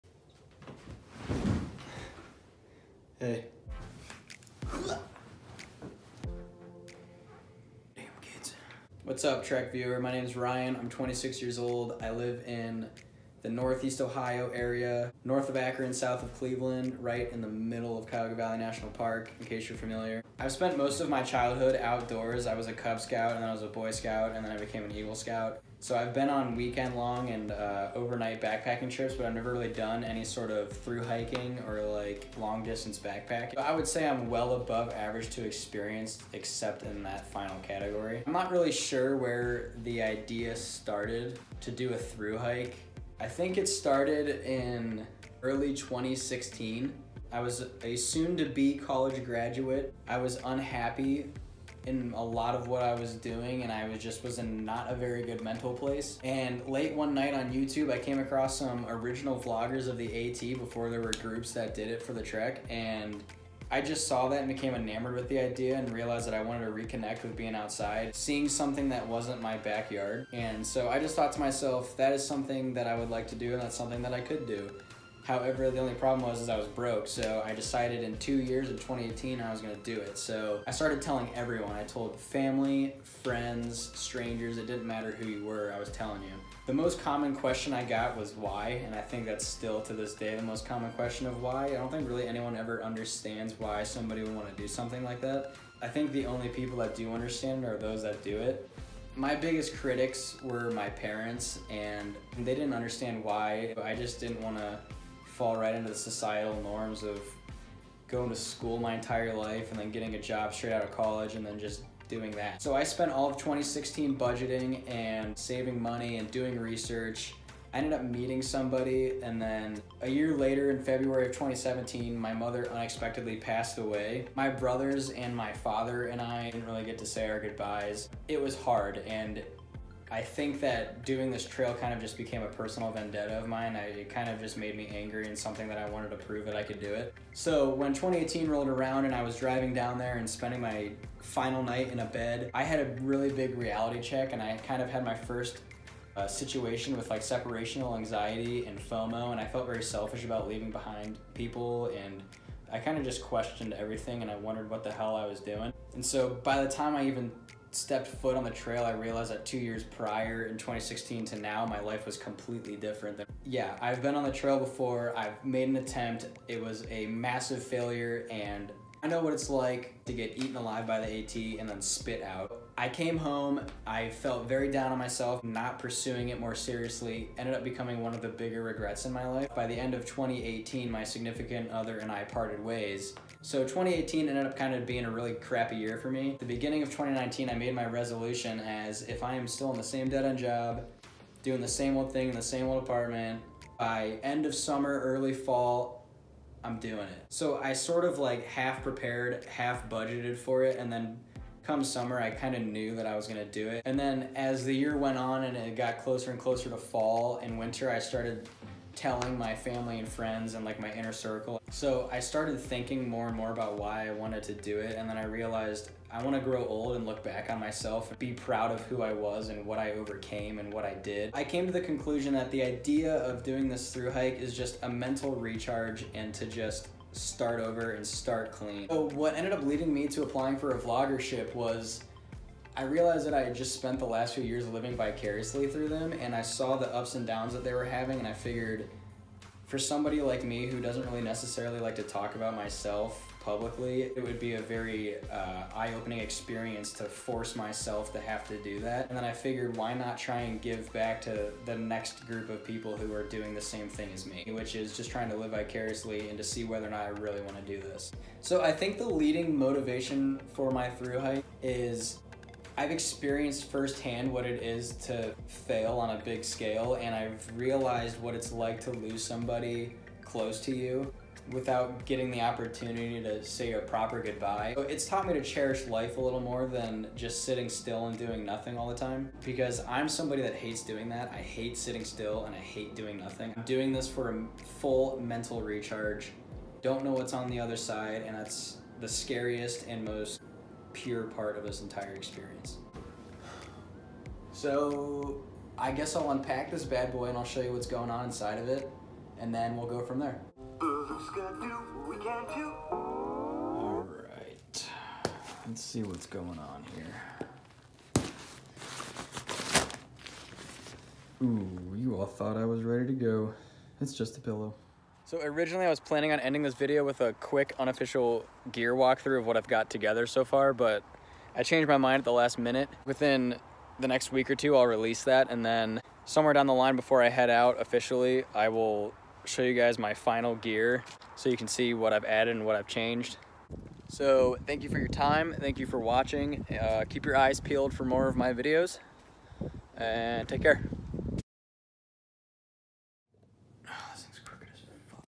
Ryan’s 2020 PRE-AT Vlog #1: Introduction
Hey Trek viewer! Thank you for stopping by and hearing why and how I’ve become an aspiring thru-hiker of the Appalachian Trail!